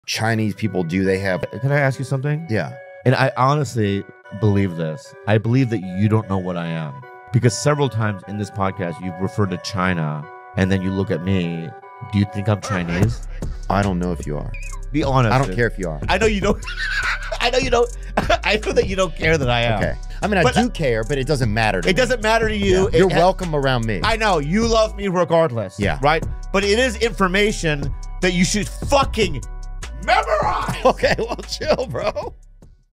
Theo Von: Bobby Lee's Not Chinese! 😂
Laugh along with Theo Von's amusing blunder as he mistakenly thinks Bobby Lee is Chinese.